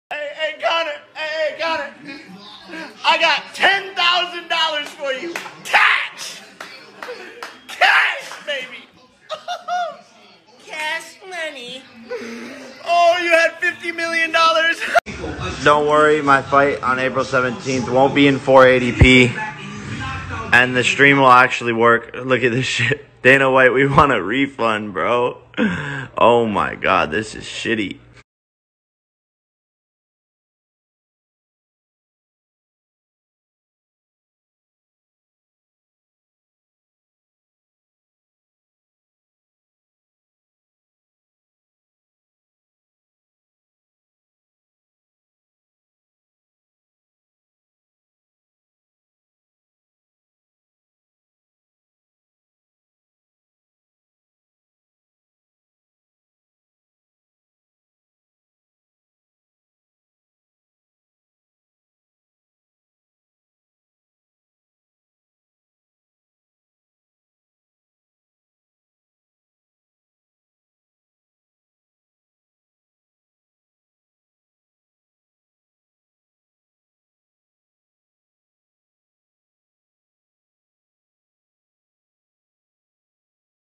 Jake Paul REACTS to Conor McGregor getting KNOCKED OUT and changes price from 50M to 10K
Credit👇👇👇👇👇
Jake Paul